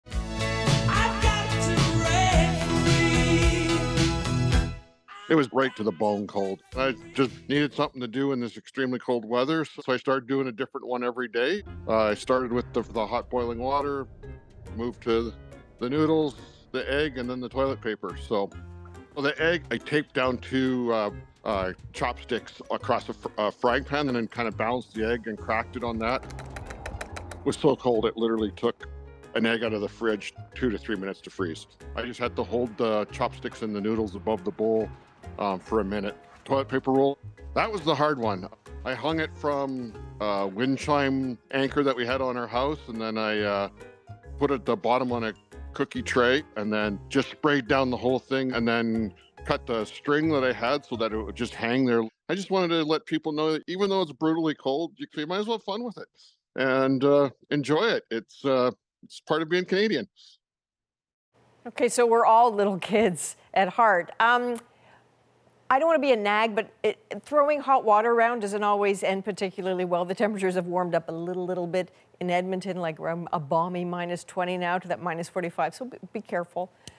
TheMoment an egg froze midair in Alberta's extreme cold
With weather in Alberta dipping as low as -45 C this week, one photographer decided to test how many items he could freeze midair.